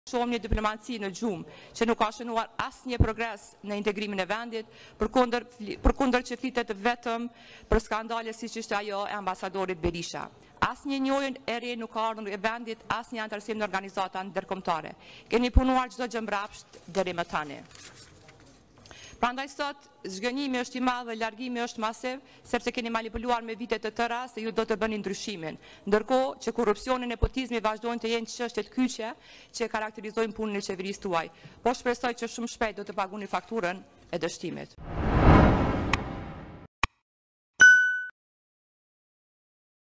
Krasniqi: Diplomacia në gjumë, çdo gjë e ka bërë mbrapsht
Deputetja e Lidhjes Demokratike të Kosovës, Rrezarta Krasniqi ka deklaruar se diplomacia kosovare është në gjumë dhe se çdo gjë që ka punuar e ka bërë mbrapsht.